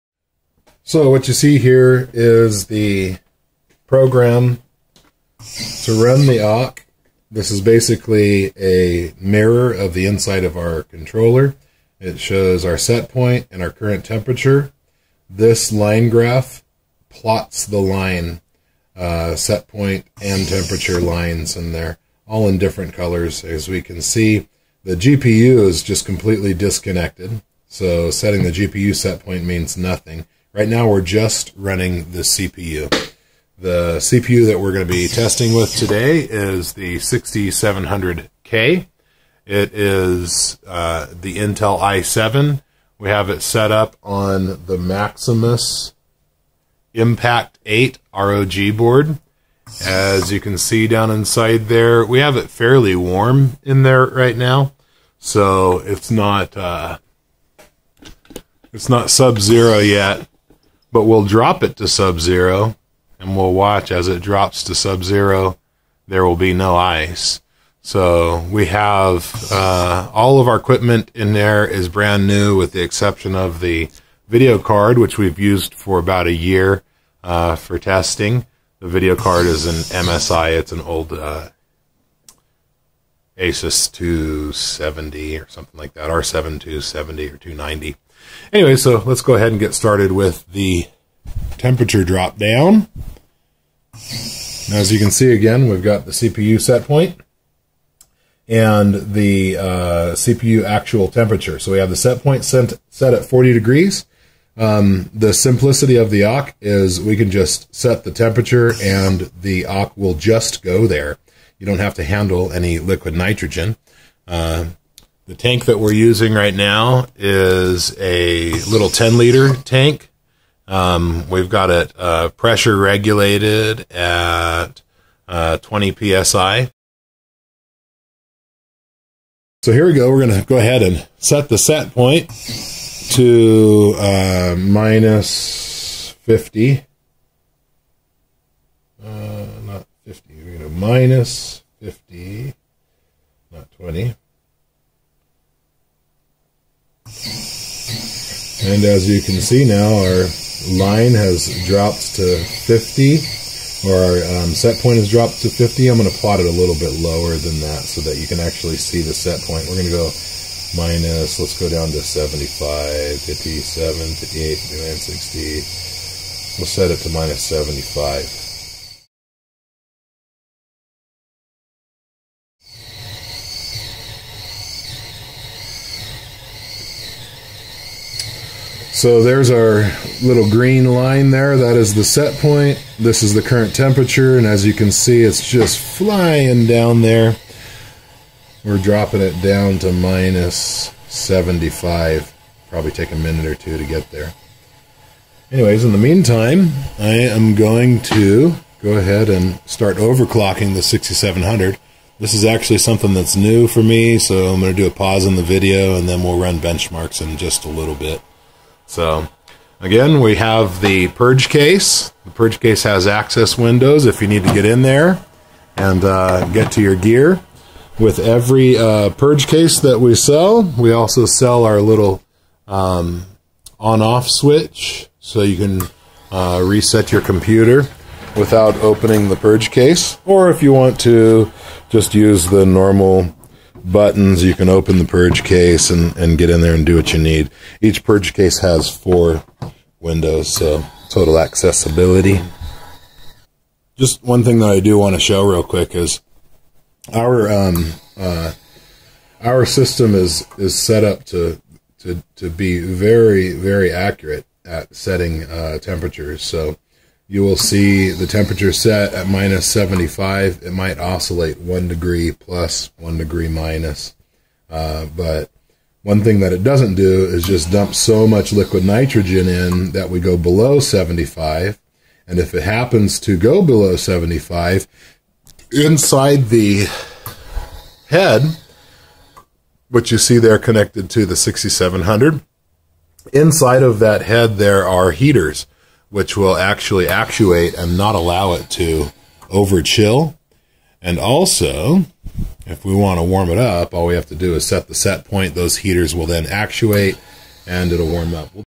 Overclocked Intel i7 6700k ASUS Maximus VIII on LN2 With OCC Extreme Liquid Nitrogen Chiller Vid#1
Video shows 6700k setup with OCC LN2 Chiller.  Showing initial Chill to -75C.  Next video shows chill to -190 C and subsequent warmups.